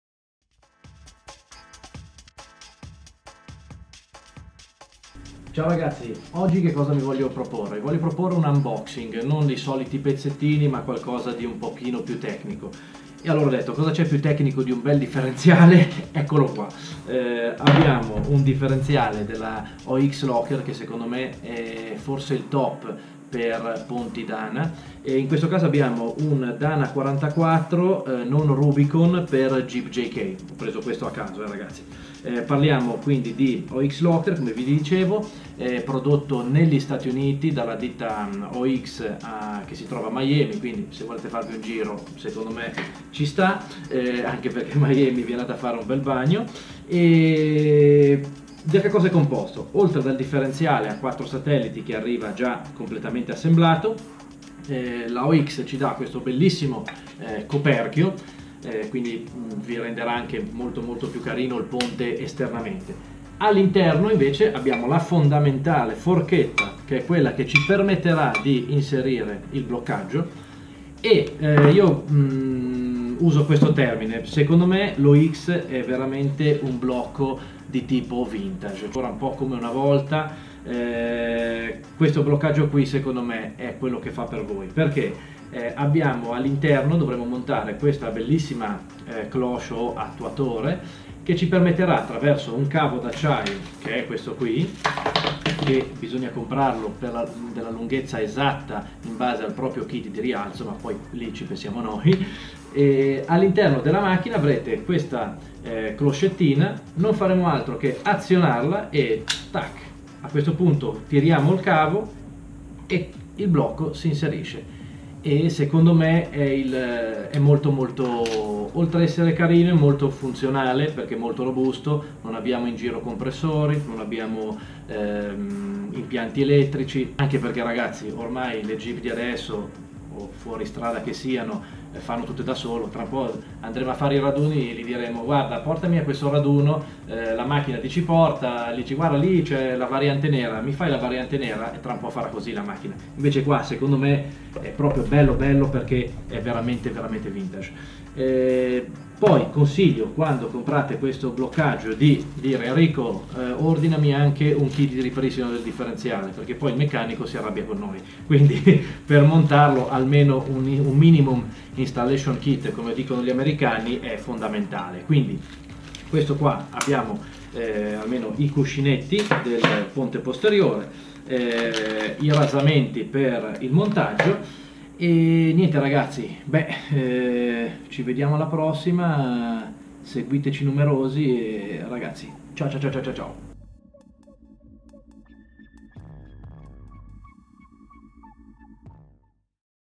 Recensione Blocco Ox Locker
unboxing blocco Ox locker per ponti dana by Performance4x4 Milano.
Una rapida video recensione di come è composto e di cosa comprende il miglior blocco differenziale per ponti Dana.Nel video andiamo ad analizzare il modello per ponte Dana 44 non Rubicon Jeep jk nelle versione con blocco 100% a cavo